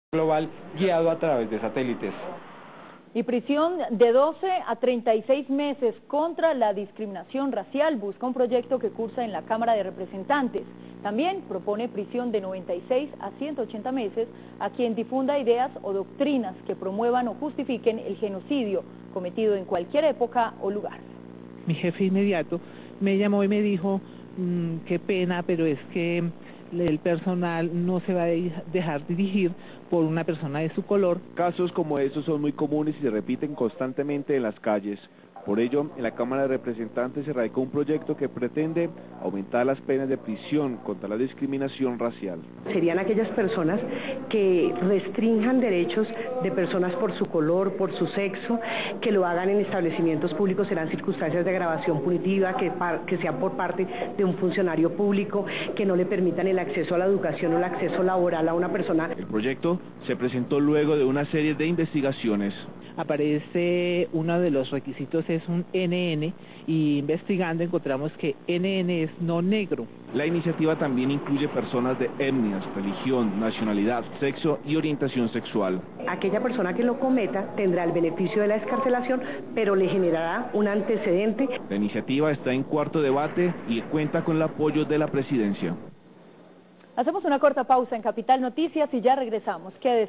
Racismo,Gloria Stella Diaz
Proyecto de Ley que busca penalizar el racismo en Colombia.